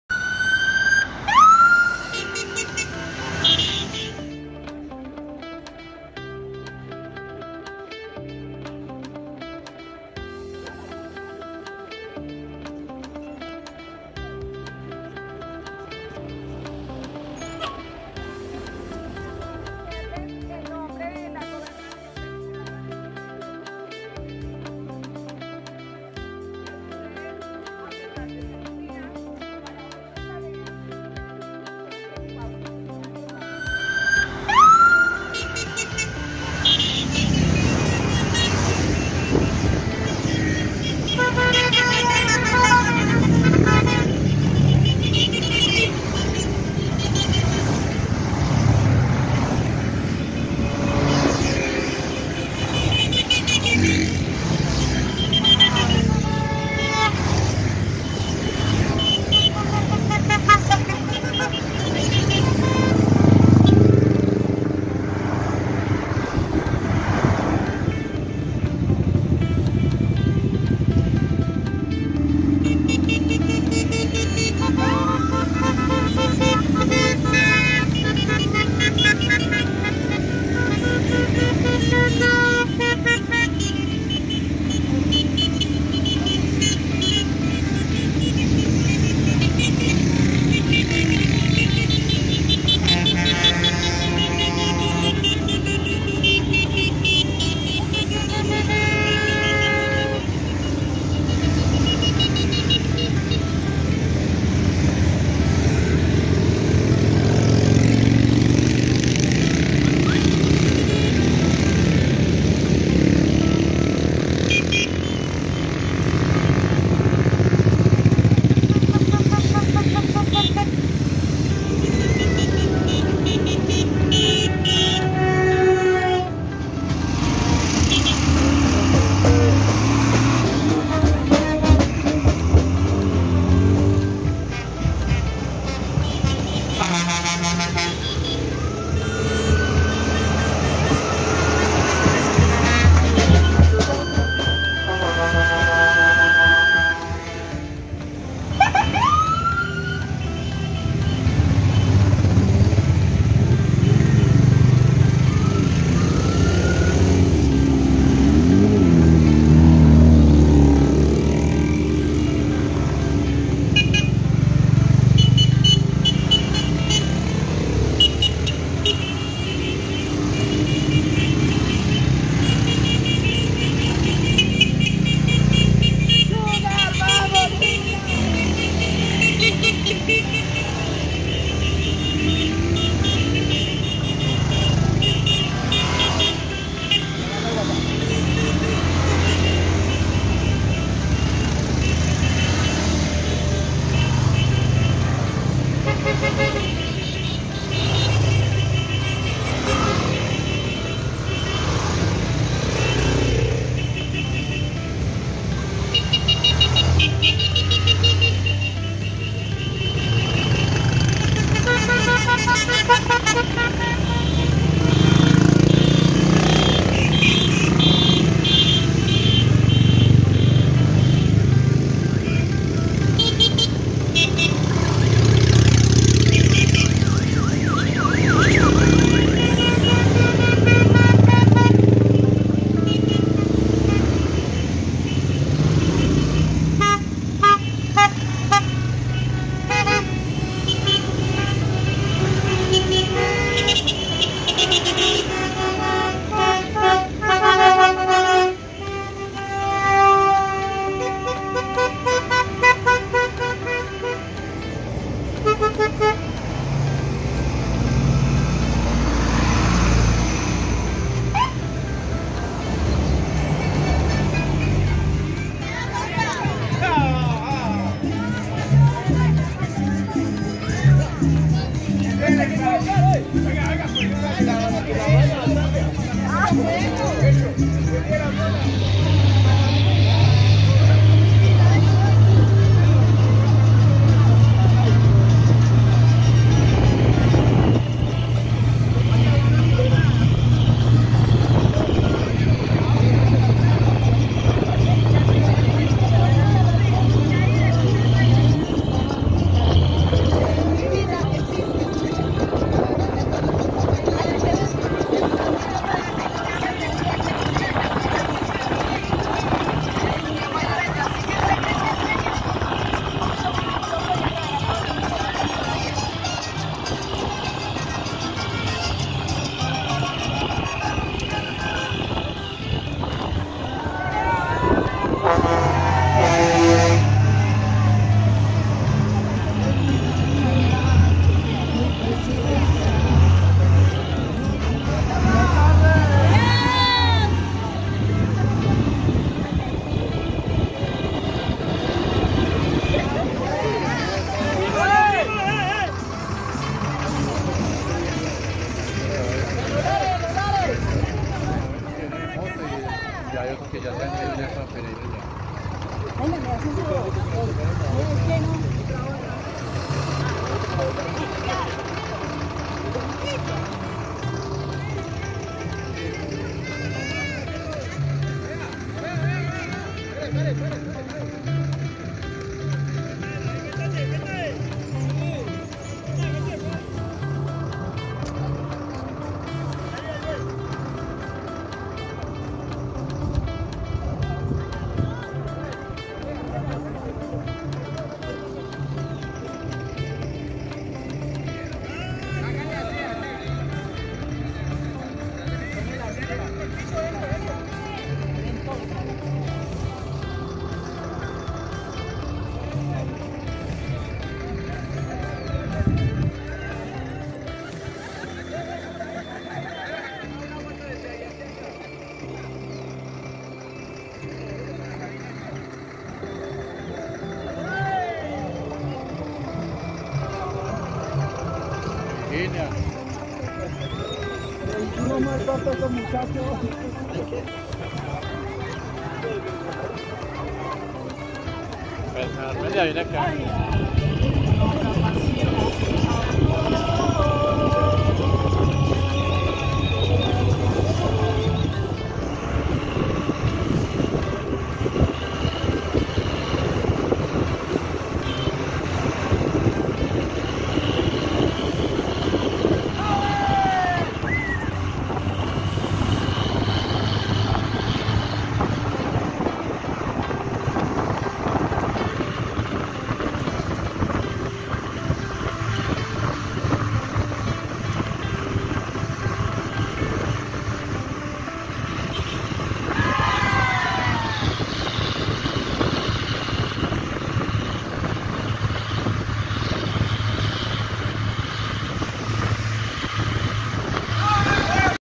Caravana turística de Pereira a Balboa
Fue una hermosa experiencia la vivida en la caravana turística desde pereira hasta balboa recorriendo la gran belleza del paisaje cultural cafetero 🌄😍🇨🇴